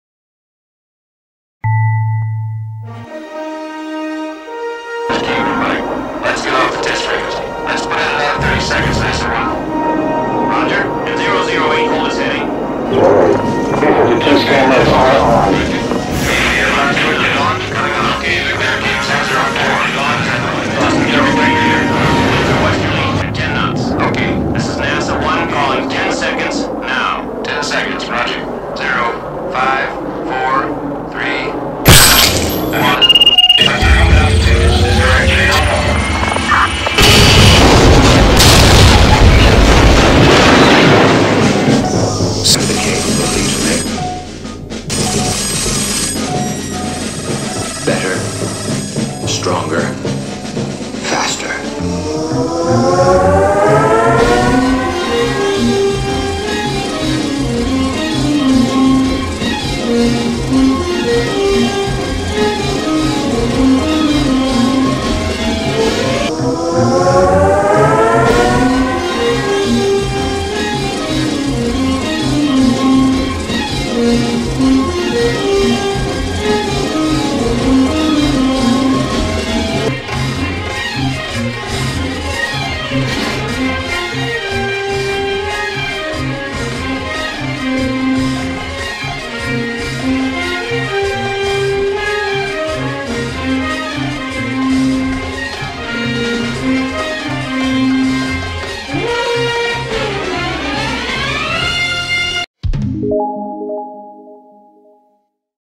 The 60 Million Dollar Woman. She has it all. Beyond Plastic Surgery.
Sixty Million Dollar Woman, Airforce Pilot, Near Death, She is Transformed Into One of The Most Beautiful super-human Women in the World.